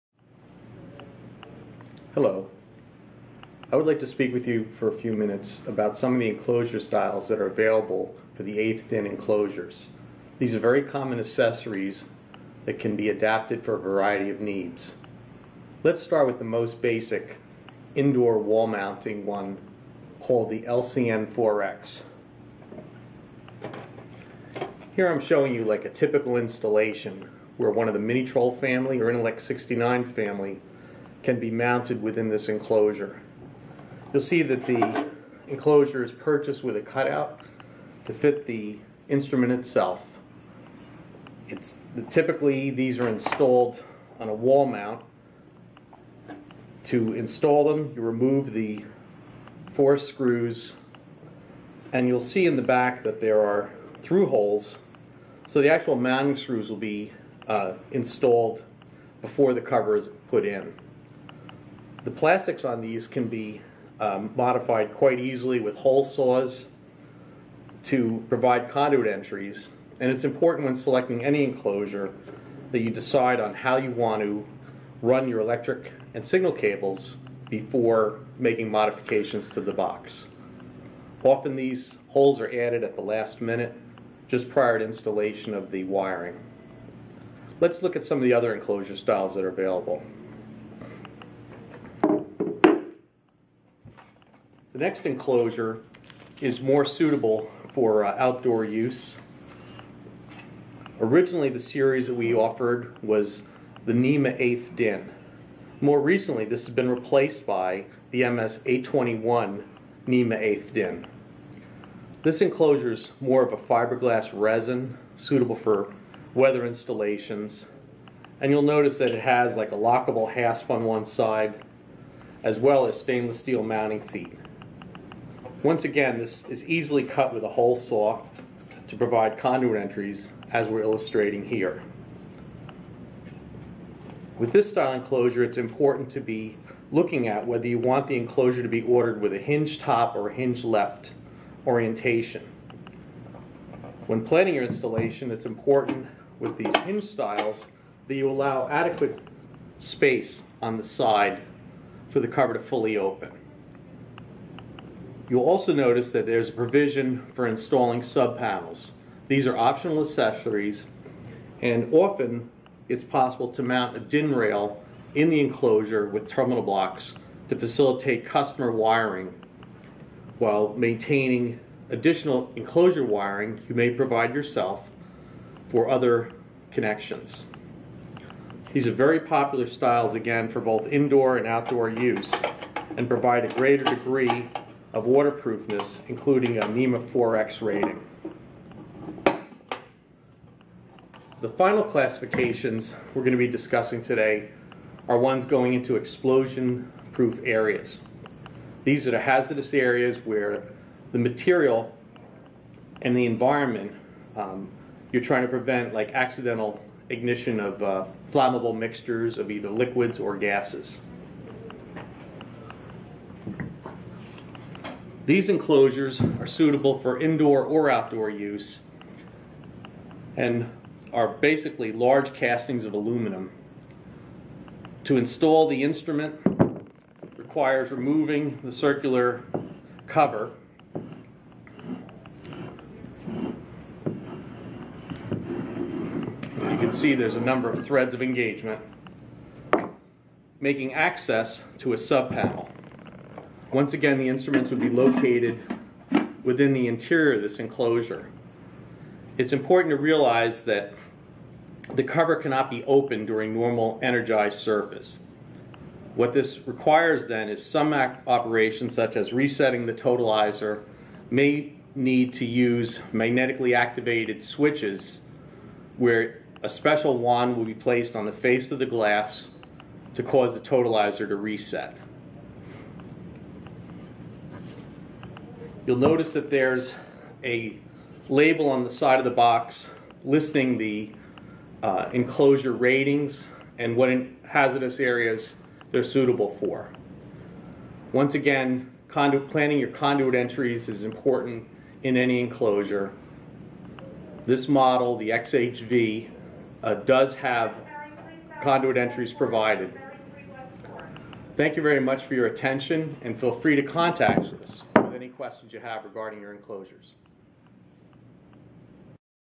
KEP NEMA Enclosures for 1/8 DIN Cutouts
These KEP NEMA enclosures offer maximum protection for your standard 1/8 DIN instruments. These enclosures meet all NEMA 4X and IP65 requirements. They are explosion proof, wall mounted and can be used for indoor and outdoor applications.